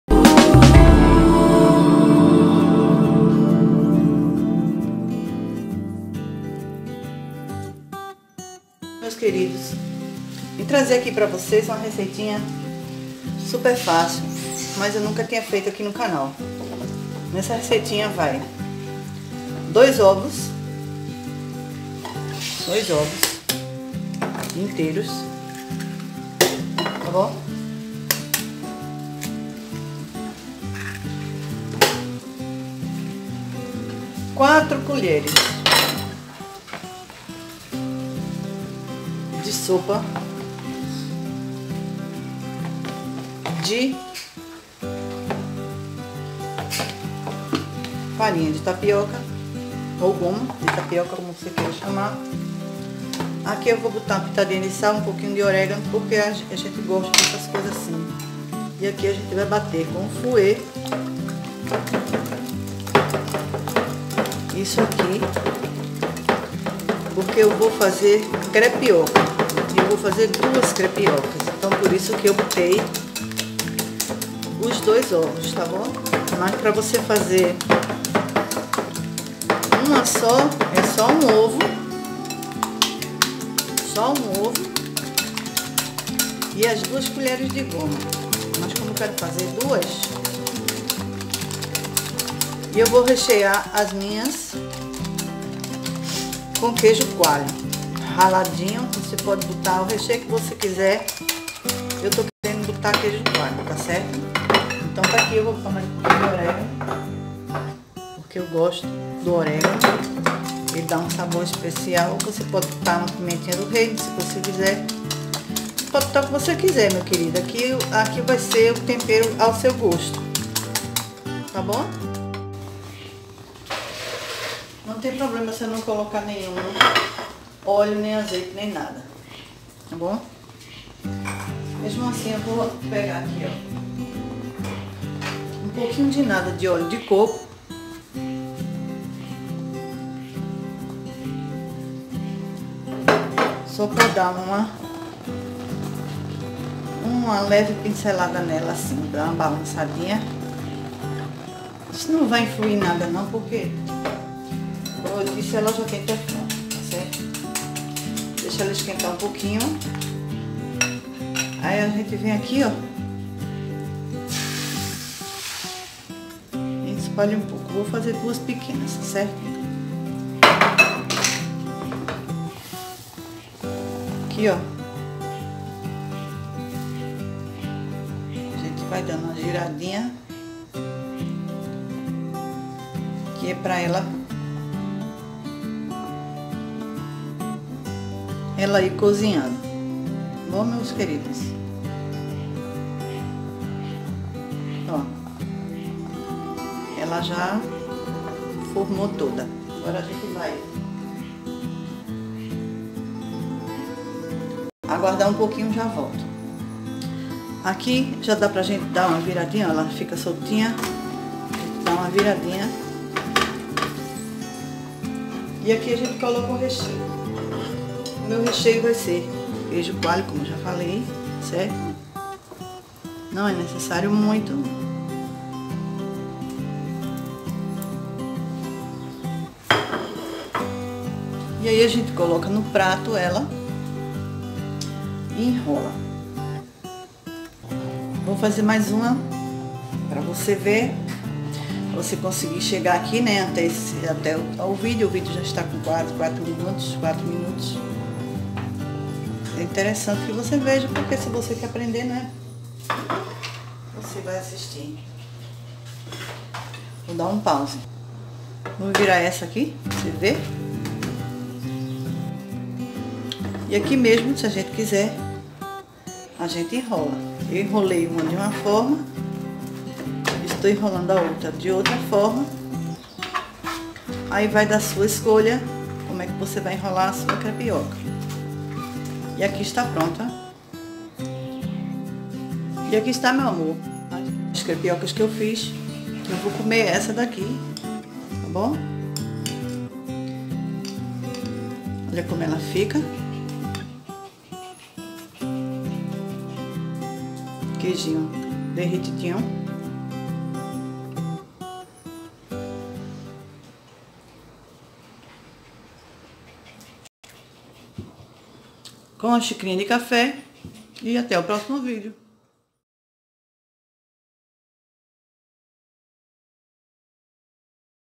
🌻CREPIOCA SIMPLES E FÁCIL DE FAZER, UMA DELÍCIA SUPER FÁCIL PRA VOCÊ
🌻Olá amigos nesse vídeo trago a crepioca simples e deliciosa, pode ser feito com vários recheios, um lanche super gostoso e prático para sua refeição, depende de sua imaginação para deixá-la uma verdadeira delícia!

Beth Garrett.

🌻 Contato para parcerias e recebidos:
(Profissional)